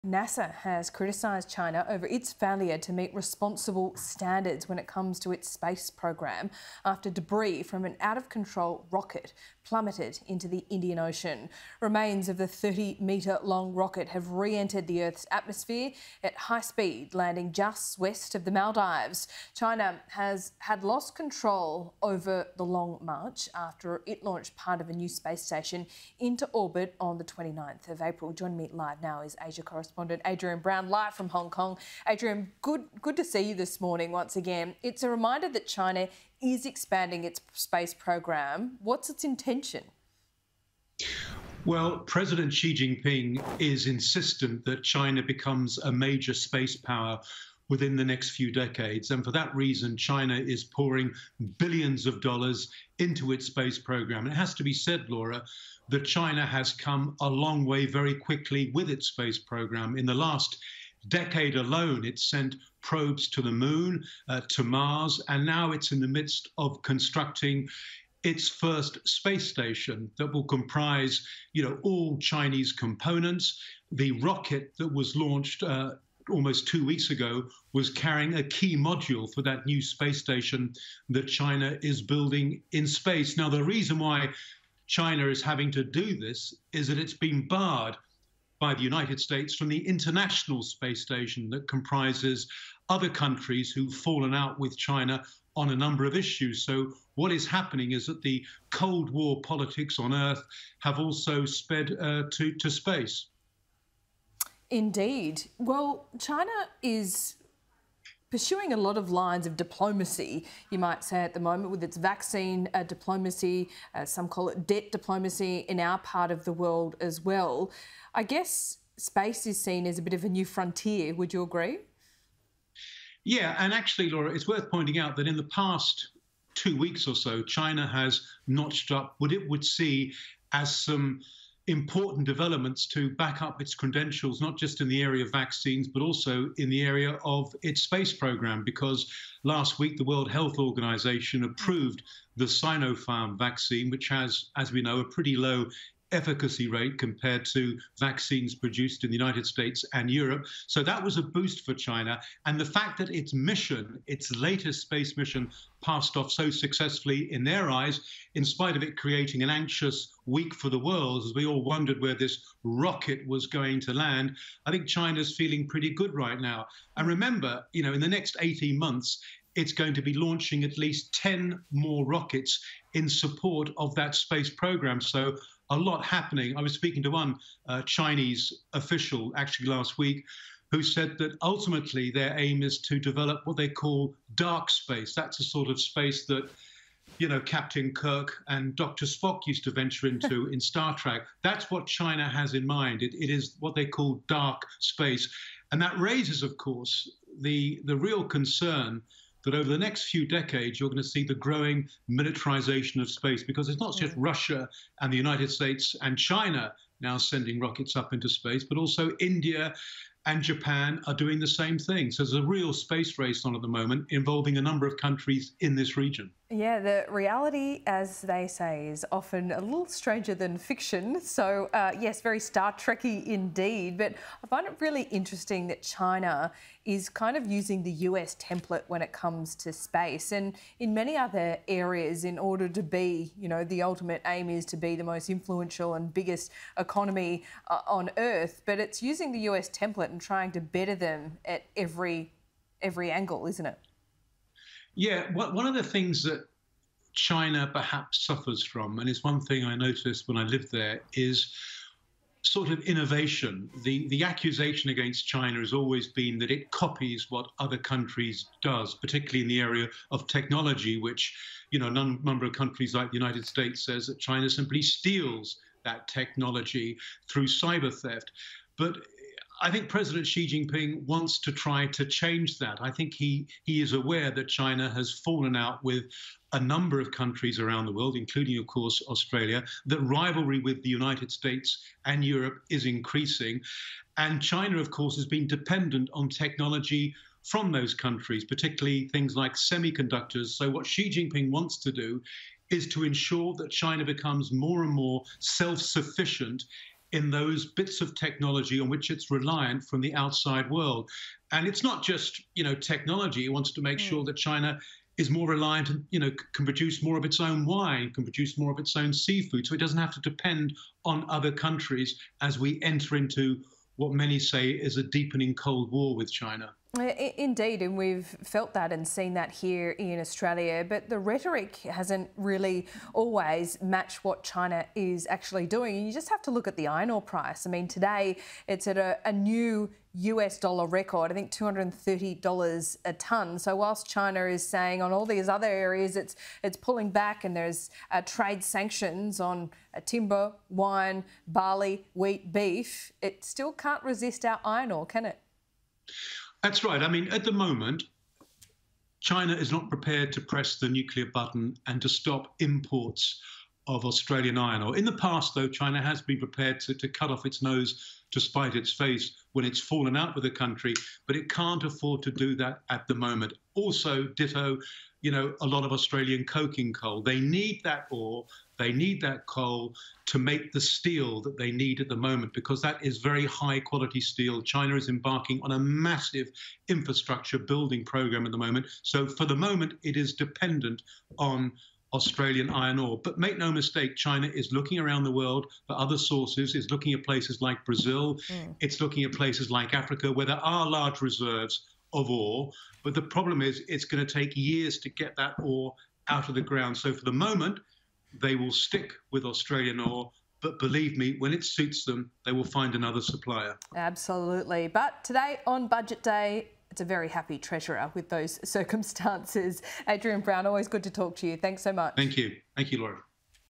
'The Cold War has spread to space' with the construction of China's space station
China is spending billions on its space program to become a “major space power” in the next few decades at President Xi Jinping’s insistence, according to Asia correspondent Adrian Brown.

“China has come a long way very quickly with its space program, in the last decade alone it’s sent probes to the moon, to Mars, and now it’s in the midst of constructing its first space station,” he told Sky News.

Mr Brown said America banning China from the International Space Station shows “Cold War politics on Earth have spread to space”.

“In the next 18 months, it’s going to be launching at least ten more rockets in support of that space station.”